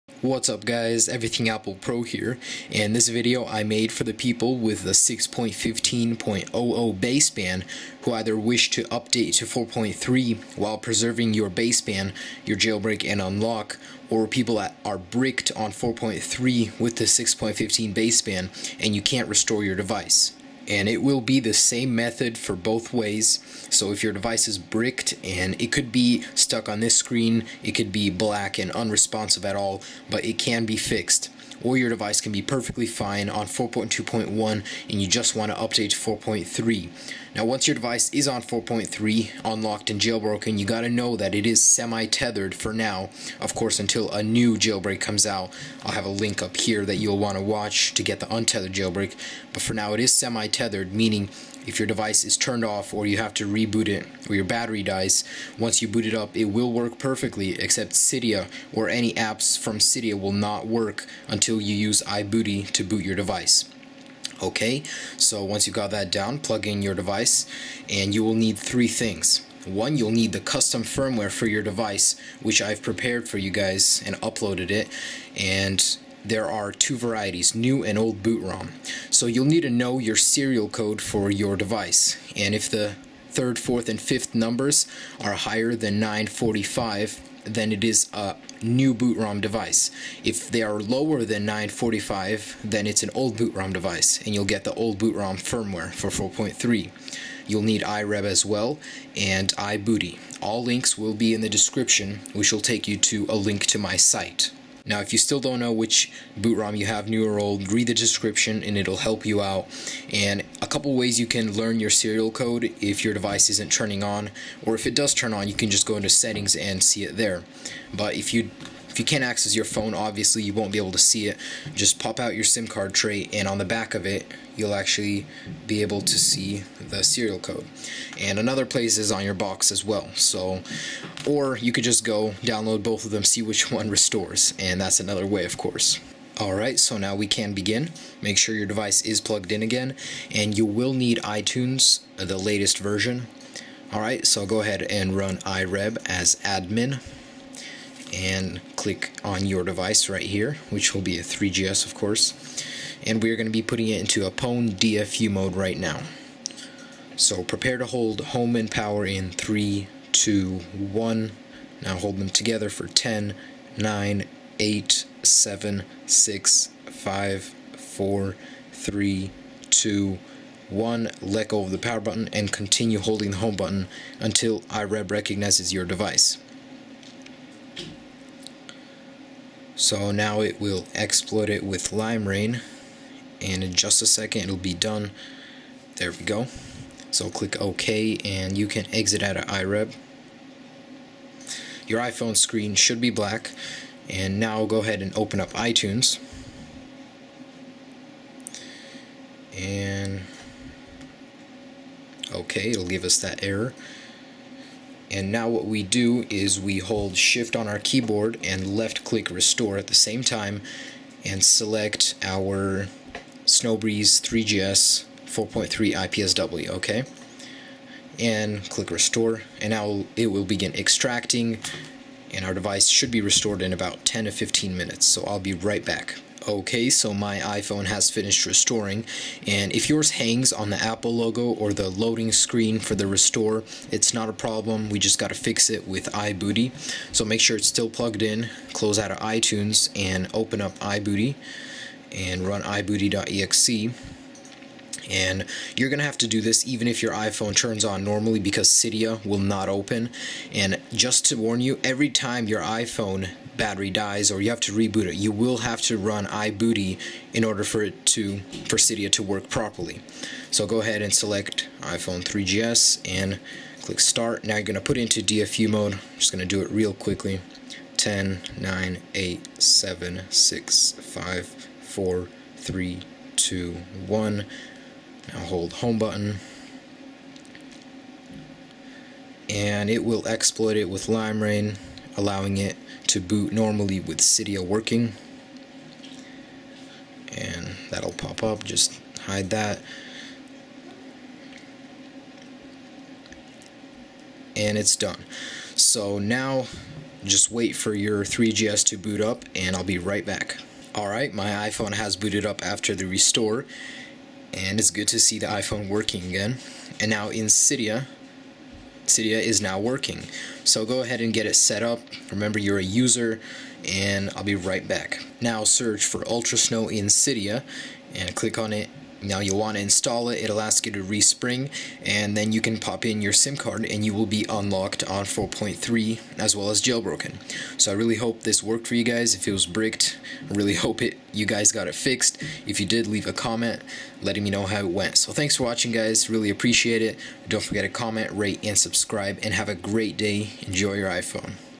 Restore & Update iPhone 3Gs To 4.3 On 6.15.00 Baseband
This Video will show you how to either fix your bricked iPhone 3Gs during the 4.3 Update or allow you to update your 4.2.1 iPhone 3Gs 6.15.00 to 4.3 While remaining Unlocked and Jailbroken (semi Tethered For Now)